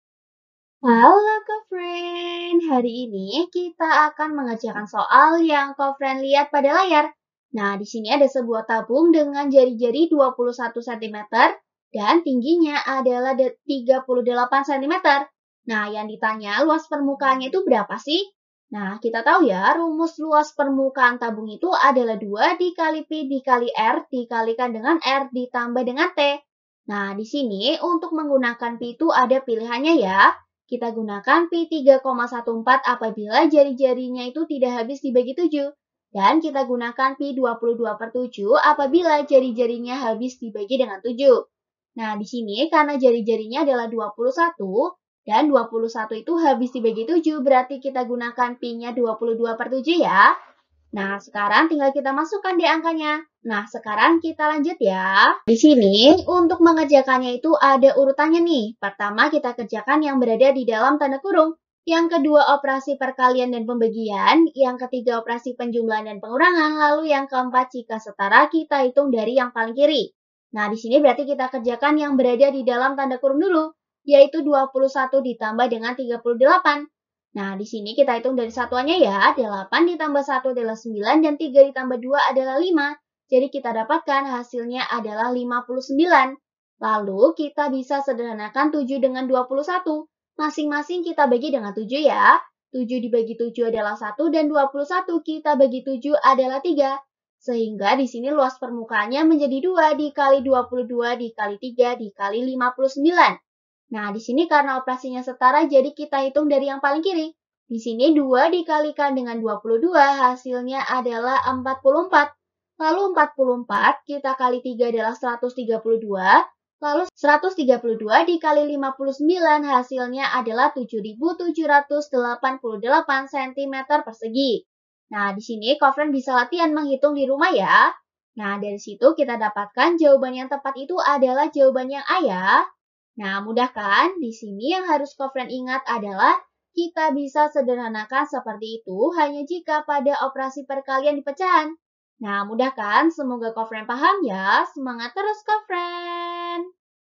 21 cm Perhatikan gambar berikut ! Luas permukaan bangun tersebut adalah cm? a. 7.788 c.7.814 38 c...
21 cm Perhatikan gambar berikut ! Luas permukaan bangun tersebut adalah cm? a. 7.788 c.7.814 38 cm b. 7.796 d 7.828

Topik atau Materi: Tabung (Luas Permukaan dan Volume) - Bangun Ruang - Geometri - Matematika Kelas 6 SD -

Sumber Soal: Matematika SD juraganles.com , N/A

00:00 : Bedah Soal
01:00 : Konsep, rumus dan pengertian Tabung (Luas Permukaan dan Volume)
01:18 : Langkah penyelesaian soal
01:27 : Jawaban akhir
01:57 : Penutup